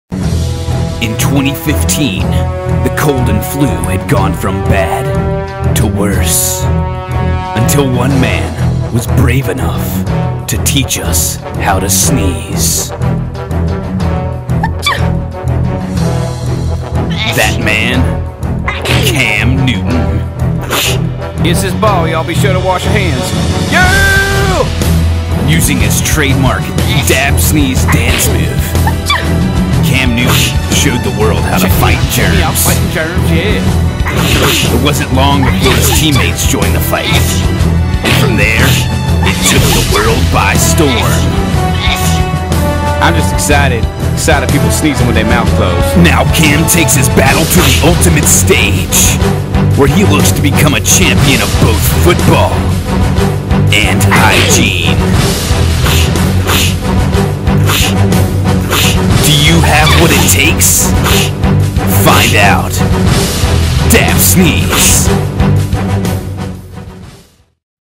Dab Sneeze
So many people think Cam dabs to celebrate when he actually dabs to help all of human kind. Watch DabSneeze and see how Cam is not only a champion of football, but also a champion of hygiene. Join Cam, stop the spread of germs and Go Panthers. Share you dab sneezing pics with dabsneeze.